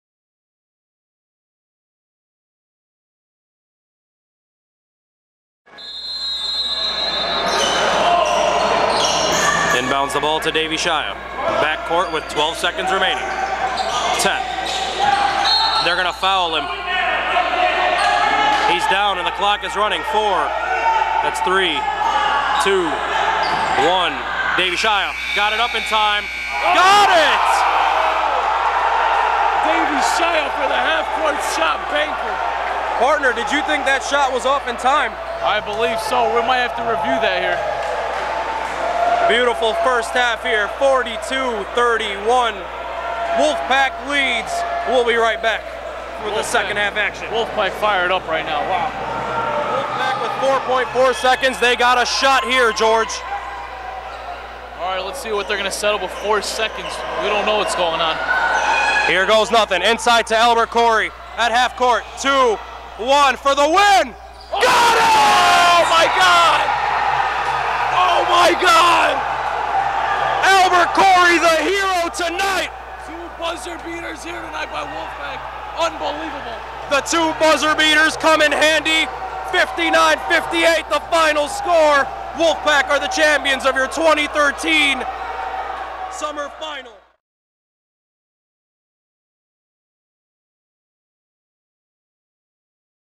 Assyrianleague.com two buzzer beaters in championship game
Assyrianleague.com - Assyrian basketball League of Chicago - David Eshaya hits the half time buzzer beater shot & Albert Khoury hits the game winning buzzer beater. Congratulations Wolf Pac on becoming the ABL 2013 summer Finals Champions!